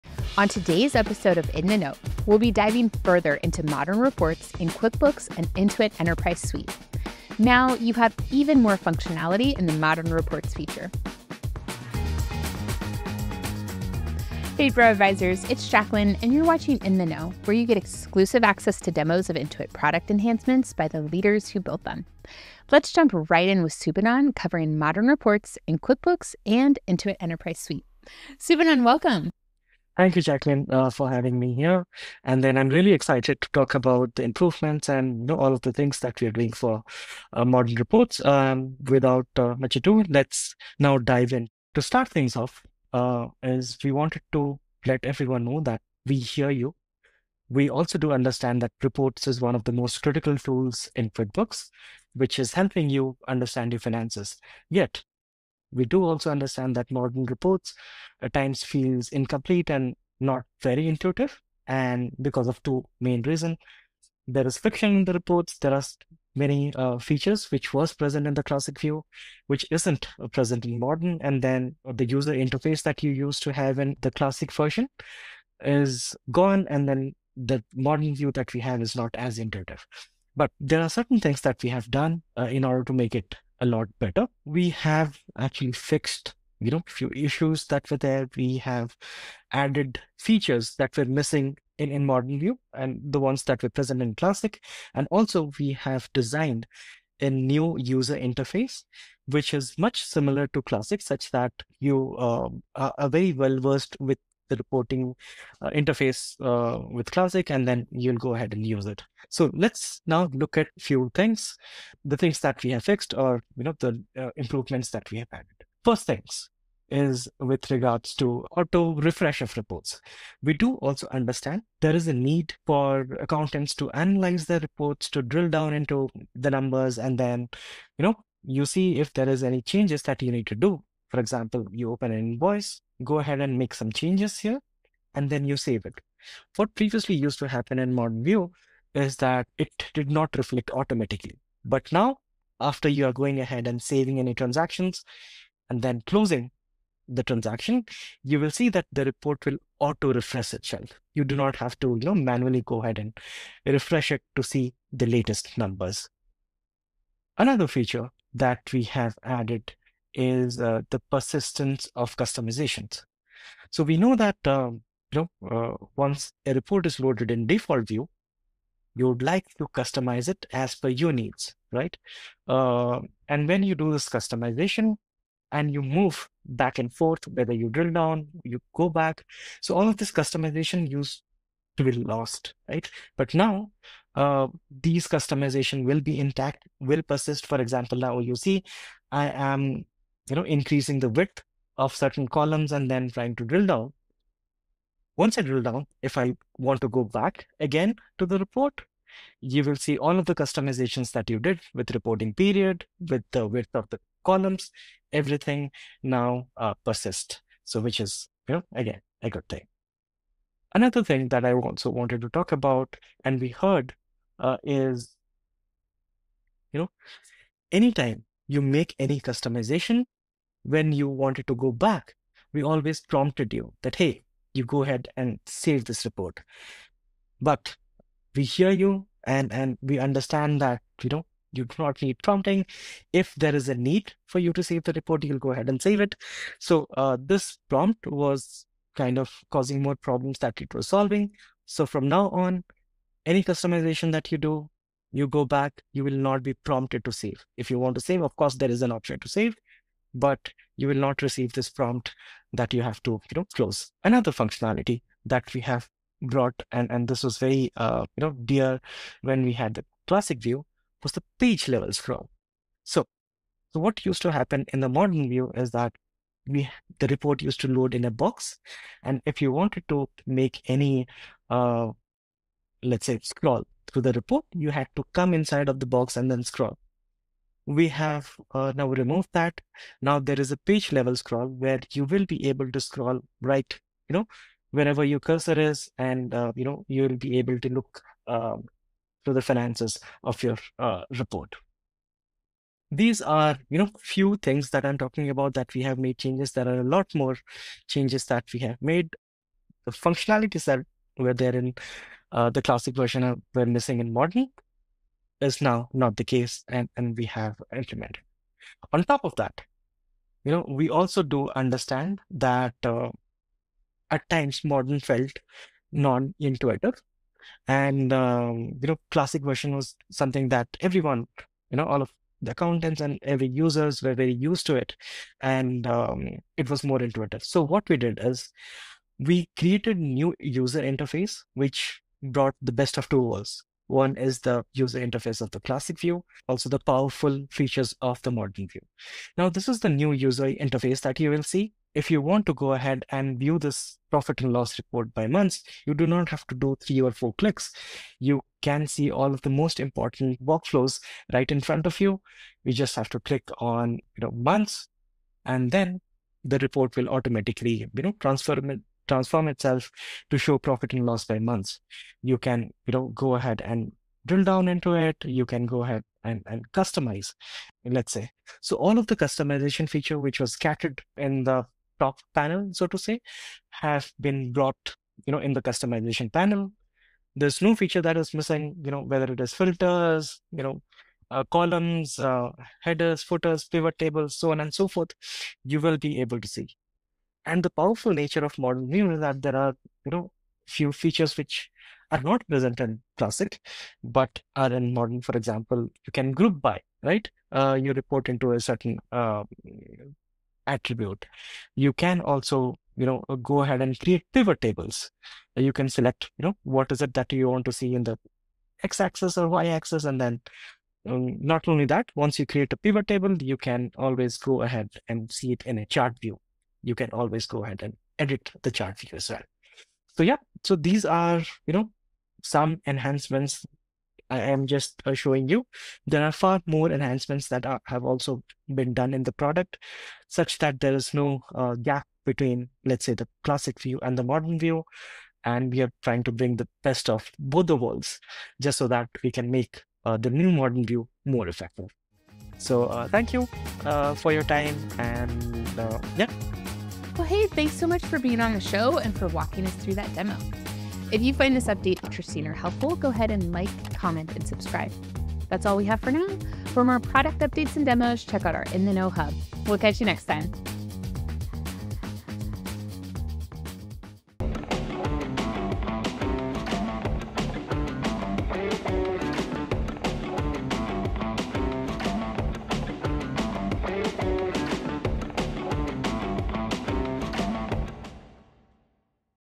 Functionality improvements to Modern Reports feature in QuickBooks and Intuit Enterprise Suite
Welcome to In The Know! Learn about the most exciting innovations across the QuickBooks Online ecosystem.

This episode is all about the enhancements to QuickBooks, and more specifically updates to Modern Reports in QuickBooks and Intuit Enterprise Suite.

Plus - you'll hear directly from Product Manager Subhanan Sahoo on how this update was brought to life.

0:00 - Introduction
0:32 - User requested updates to reports
1:56 - Reports auto-refresh after saving
2:36 - Persistence of customizations
4:14 - UI functionality improvements
5:09 - Updates to modern view interface
6:28 - Create pivot tables or view as chart
7:22 - Outro